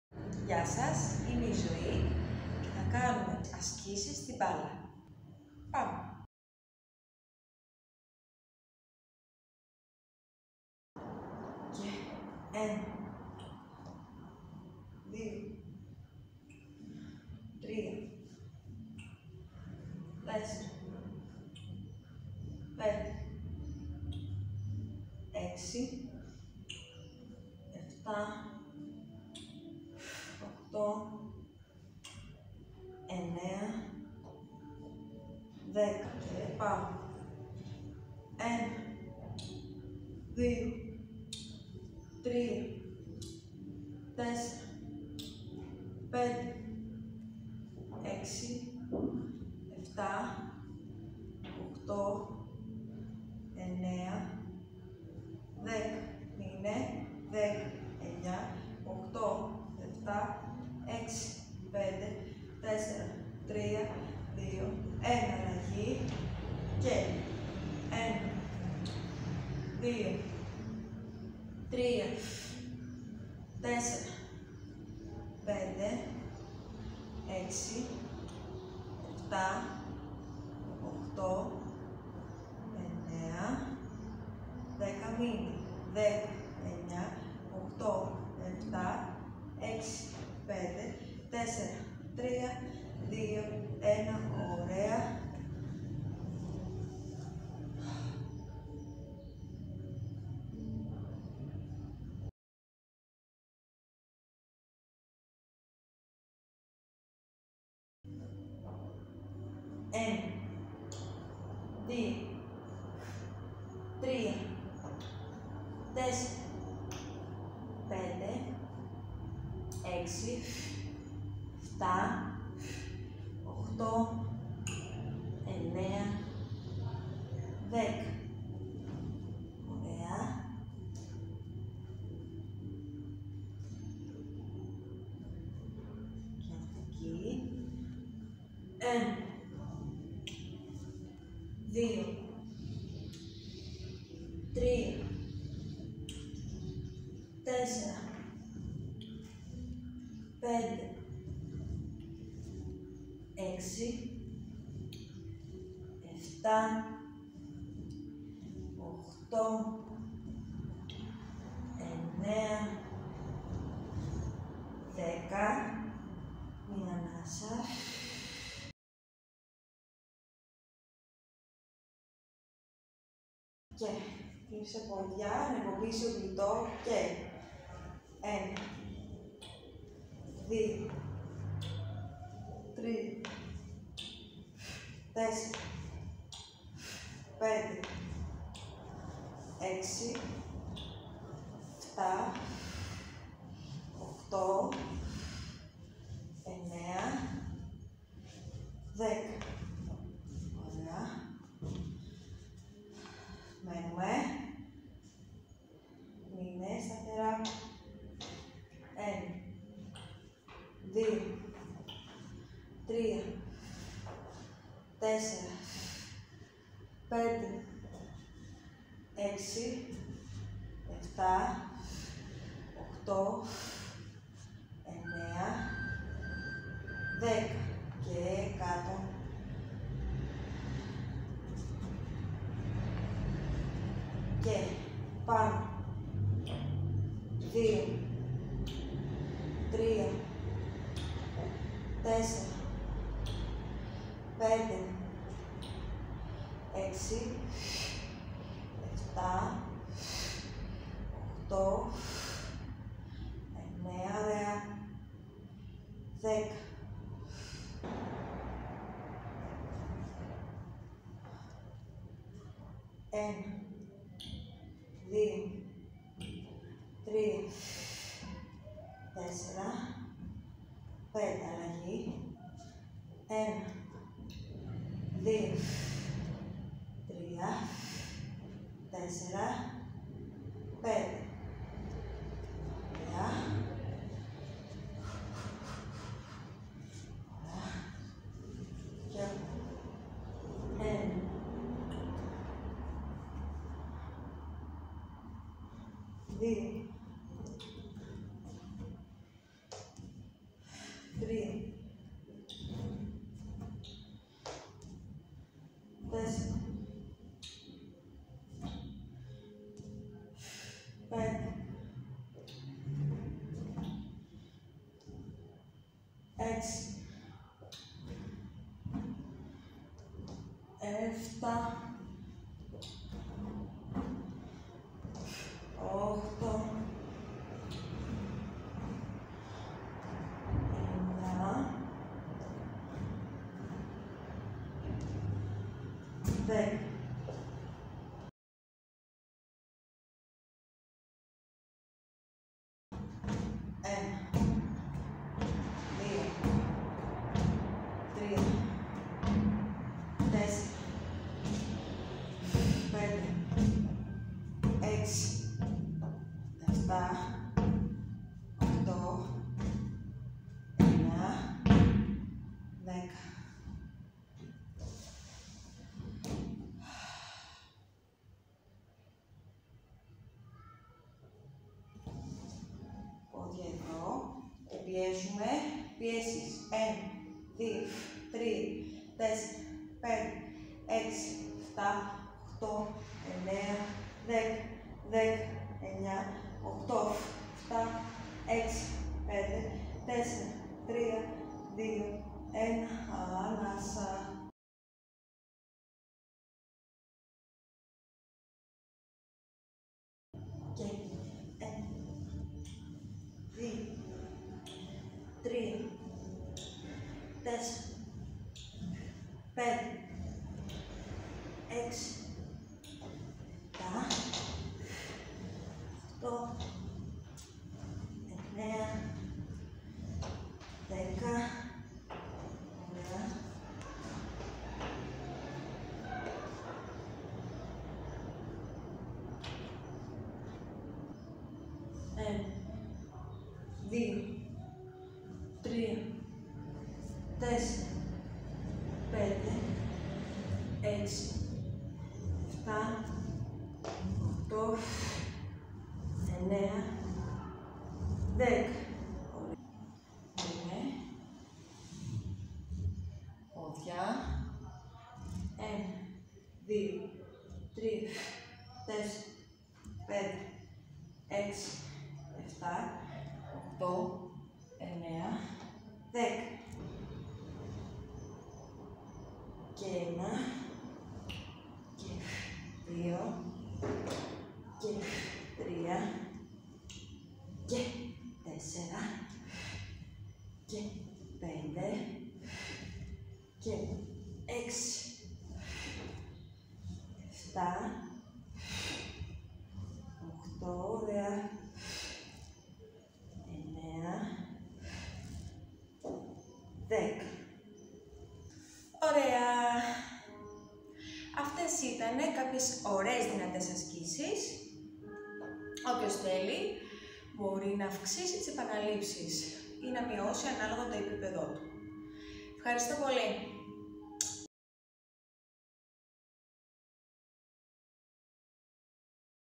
Pilates 8λεπτά γρήγορη δυνατή προπόνηση με μεγάλη μπάλα Zoe Papadopoulou
Pilates 8λεπτά γρήγορη -δυνατή προπόνηση με μεγάλη μπάλα

Η σημερινή προπόνηση είναι 8 λεπτά γρήγορη-δυνατή για ολόκληρο το σώμα με μεγάλη μπάλα.
Το μέγιστο των δυνάμεων κάθε ανθρώπου είναι διαφορετικό, γι’ αυτό αν νιώσετε αδιαθεσία καλό θα είναι να διακόψετε το πρόγραμμα ή να μειώσετε την ένταση ή να κάνετε ένα ολιγόλεπτο διάλειμμα, κατά την διάρκεια των ασκήσεων.

Προσοχή! Το πρόγραμμα απευθύνεται σε άτομα που δεν έχουν διεγνωσμένα προβλήματα υγείας.